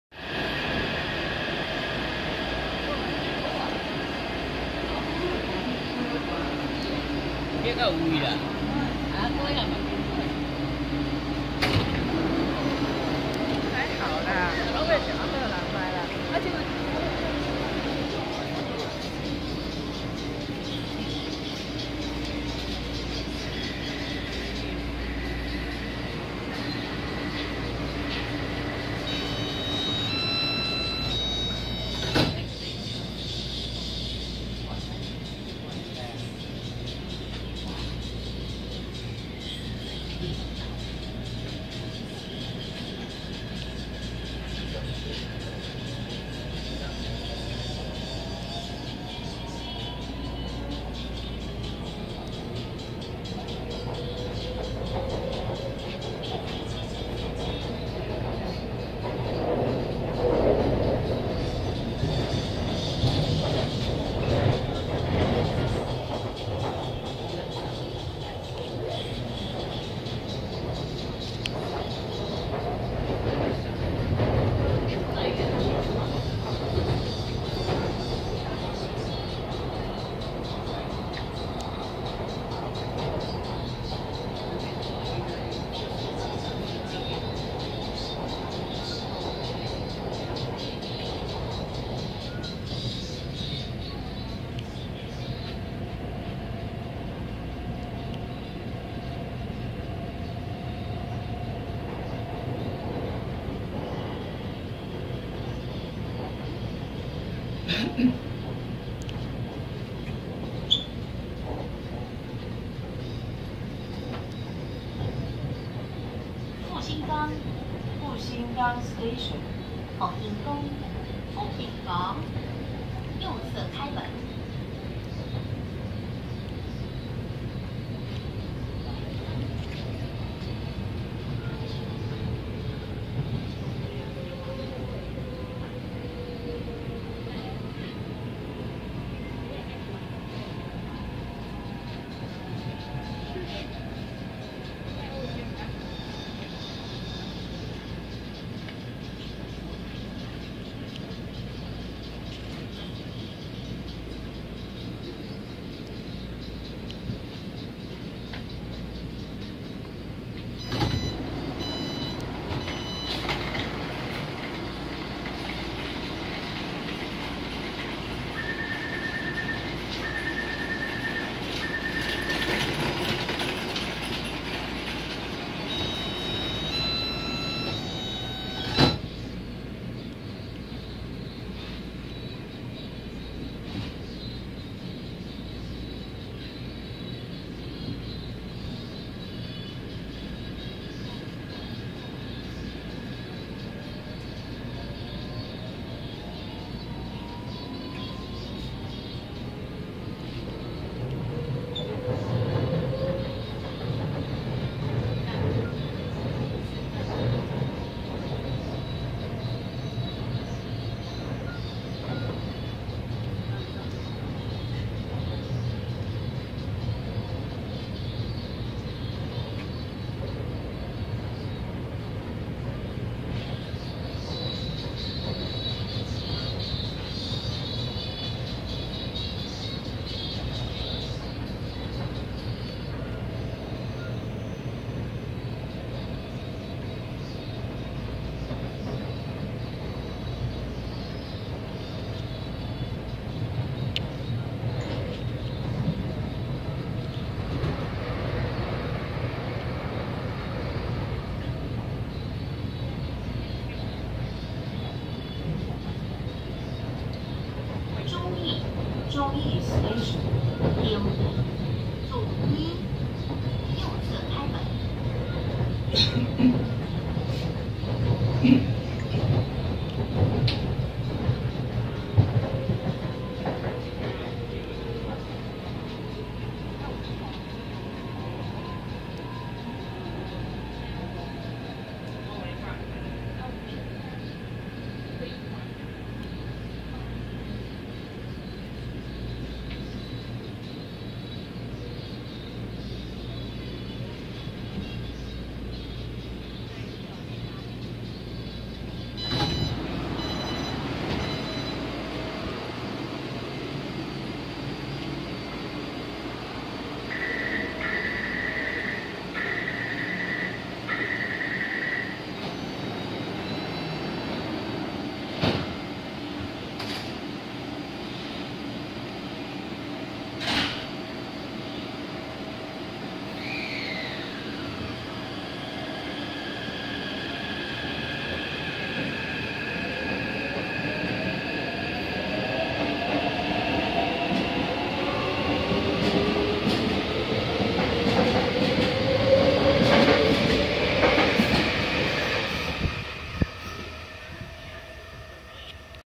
|交通迷天地| [台北捷運] 381型迪士尼米奇90周年彩繪列車 511/512 往R28淡水 R22北投~R24忠義+離站
車號: 511/512